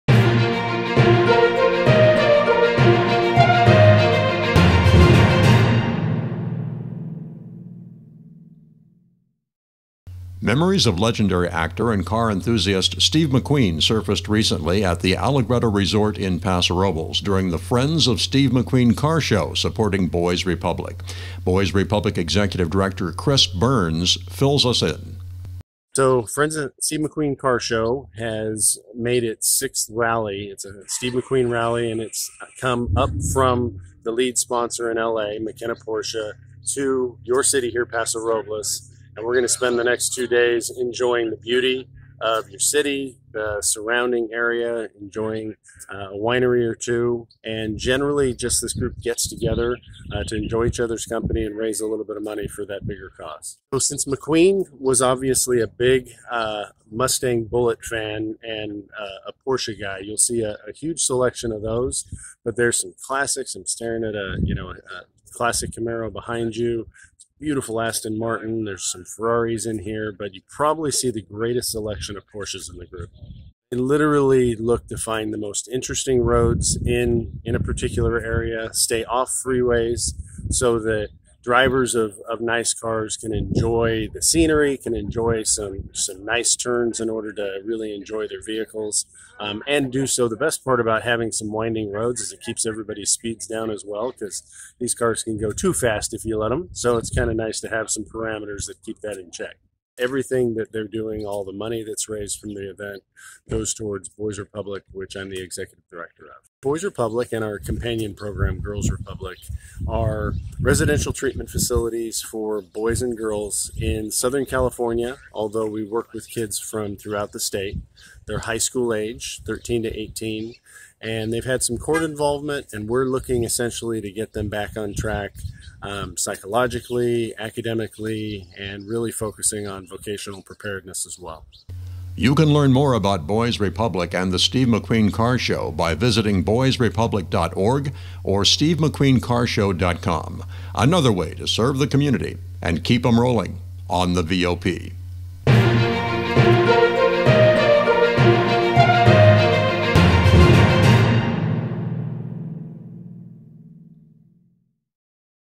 Friends of Steve McQueen Car Show in Paso Robles CA
Drivers take high-end sports cars on winding, rural Paso Robles roads to support Boys Republic.